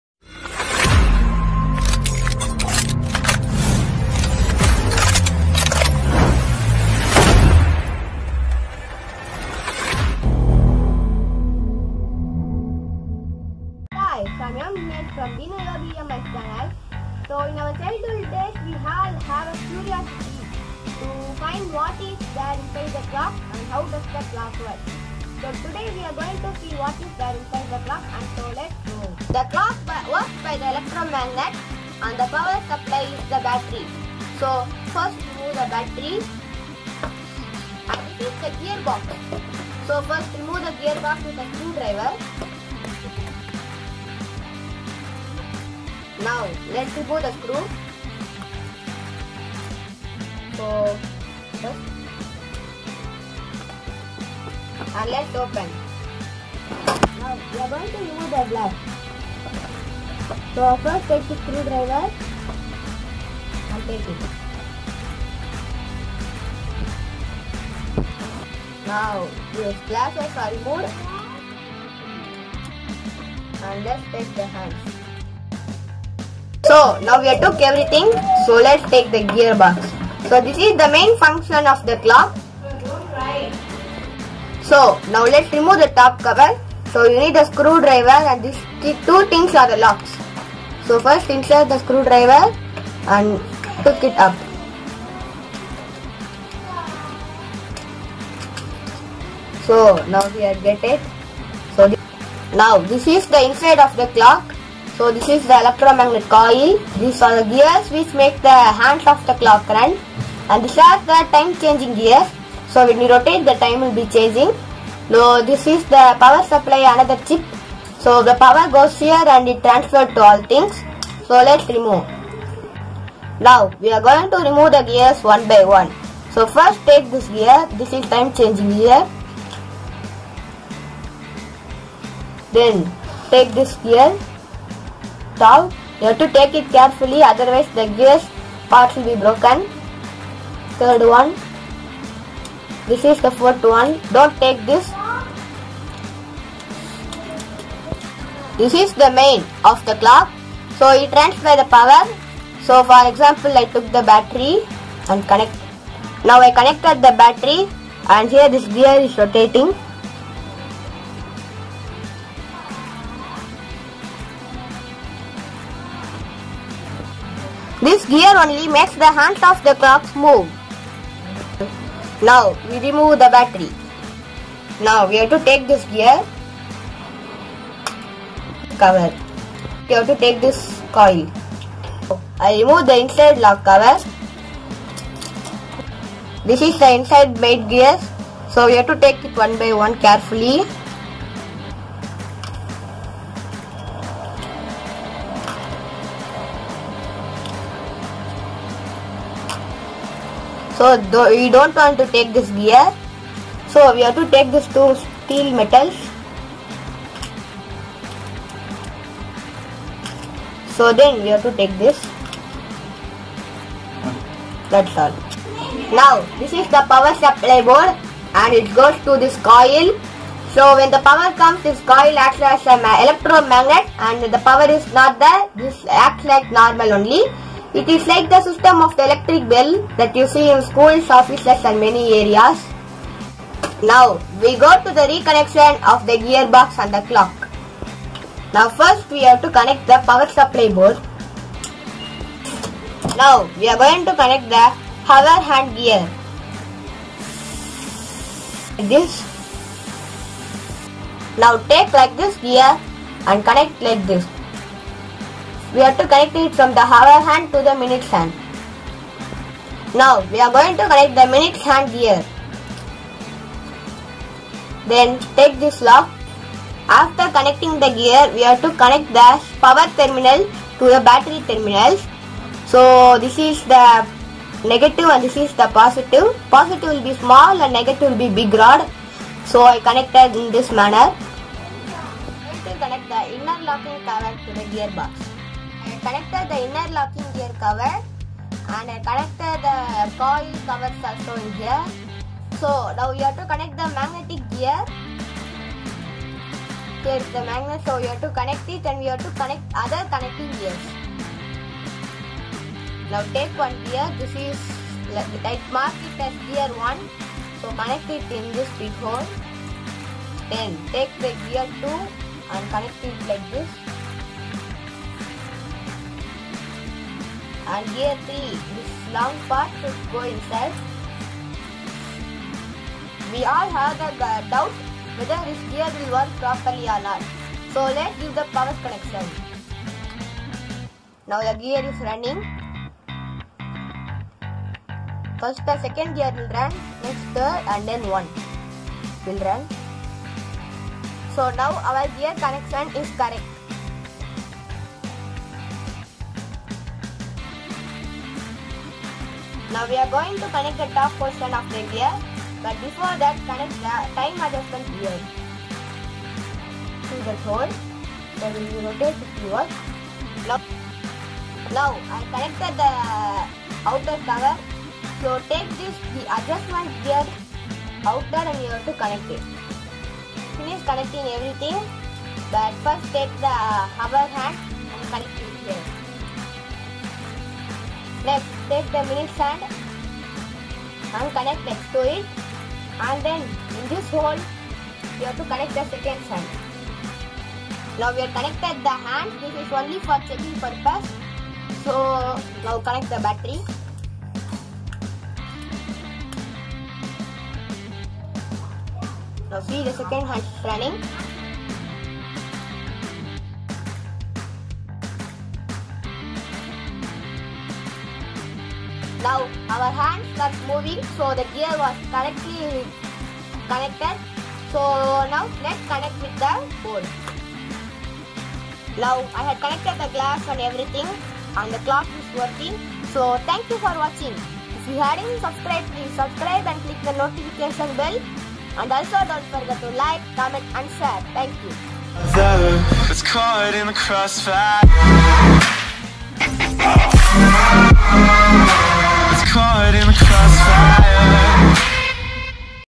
Quartz_clock assembling and dismantling | DeenuYogi MS
Hi viewers,
Everyone thinking how does the clock work? This video is a great soolution and this video is about the assemble and dismantle of a clock. We will see how does the hour hand connected to minutes hand and how does the minutes hand connected with secoonds hand.